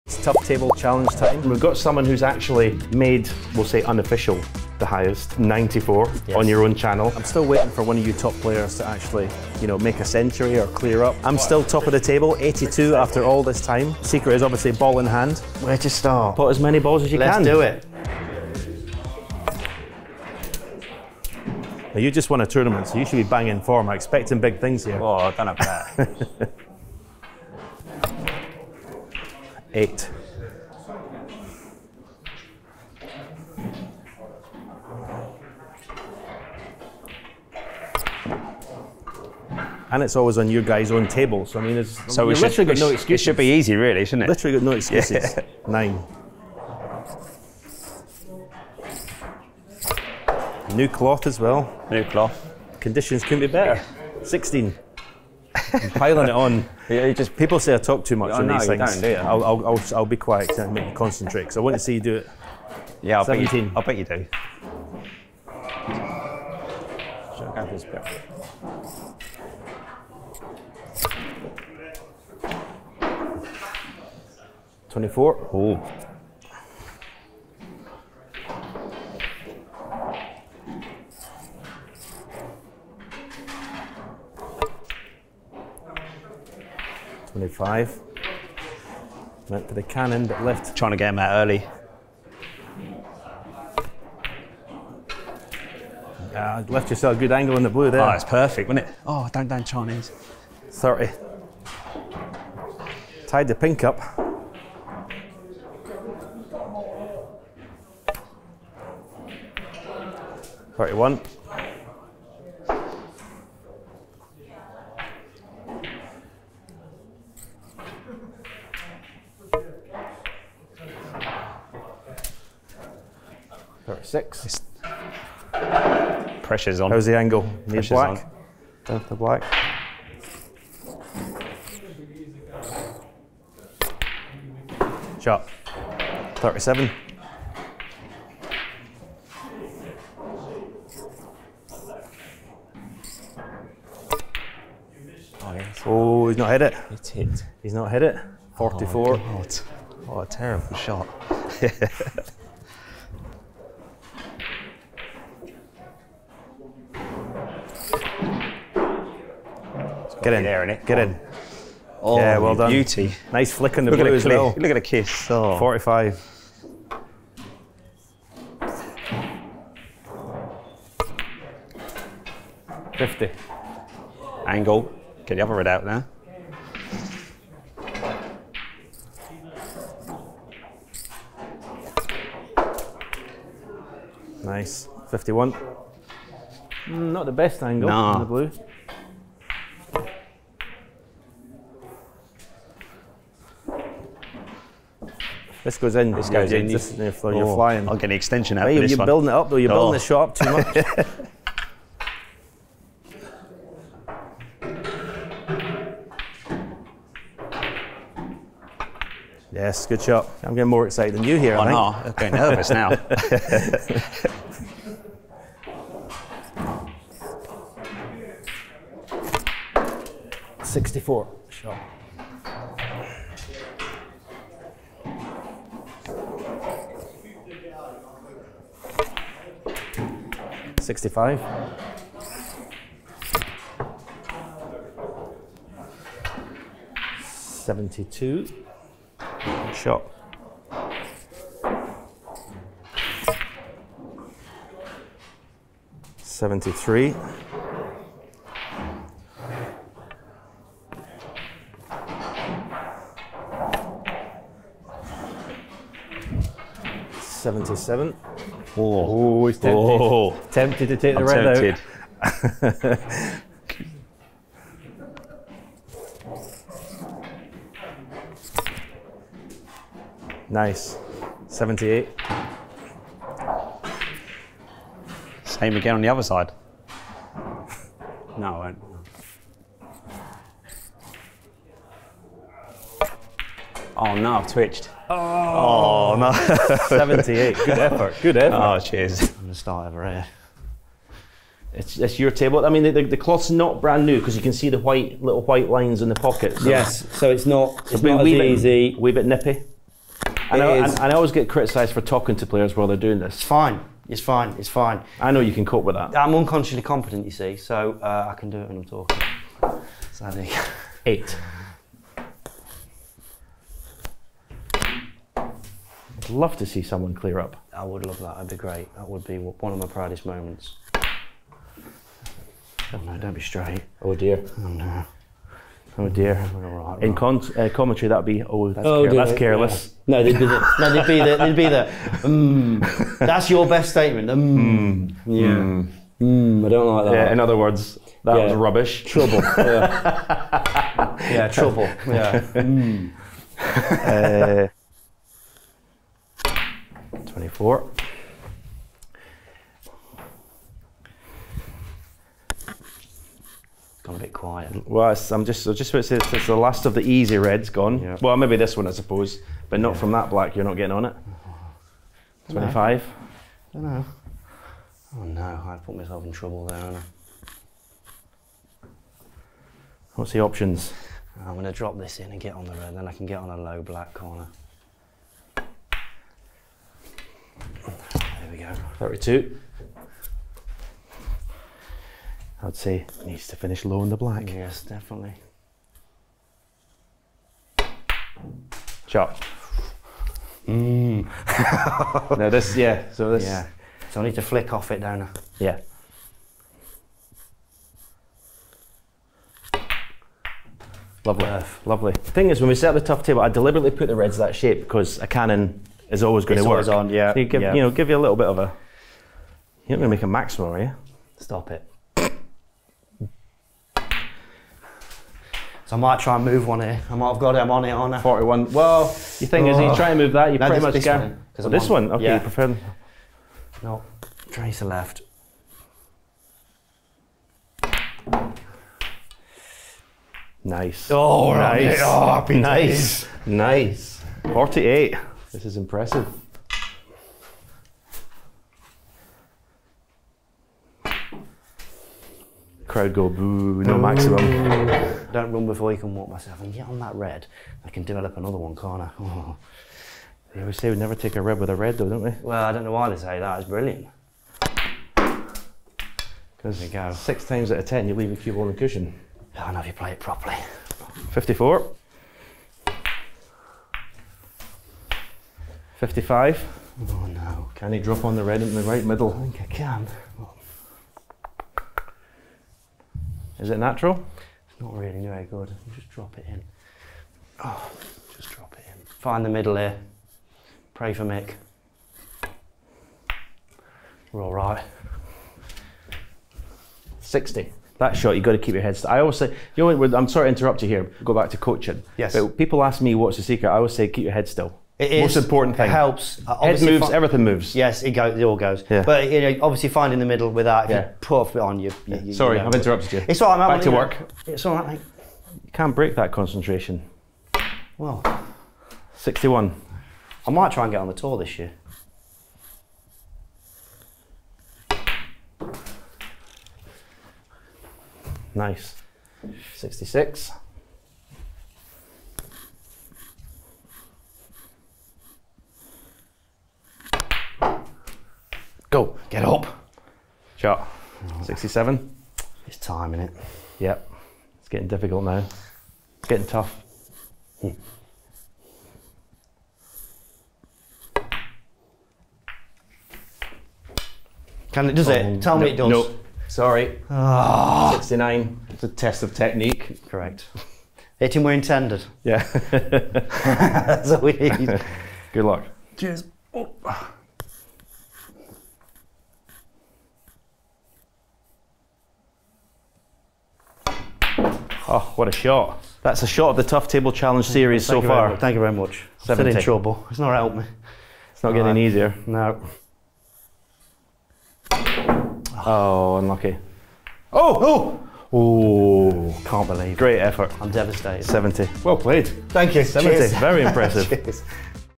The Best Tough Table Challenge Yet? (Barry Hawkins & Michael Holt)
Michael Holt and Hawkins attempt the Tough Table Challenge to try and finally beat Stephen's high score and they might be the best attempts yet!

Also, make sure you're following us on the social platforms below for even more content.